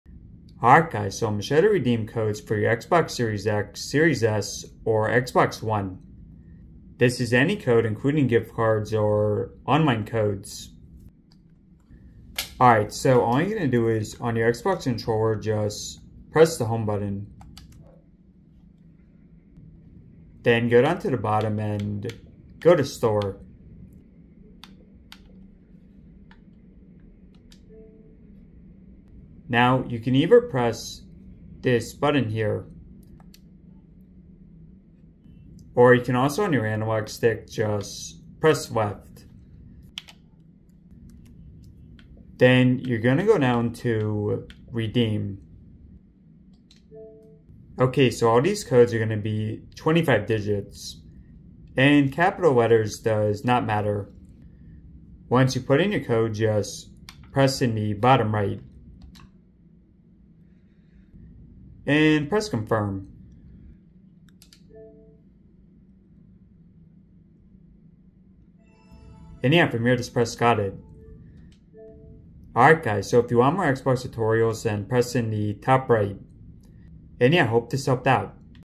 Xbox (Series X, S, One) How to Redeem Gift Card Code!
How to redeem gift cards or enter digital codes on Xbox Series X, Xbox Series S, or Xbox One.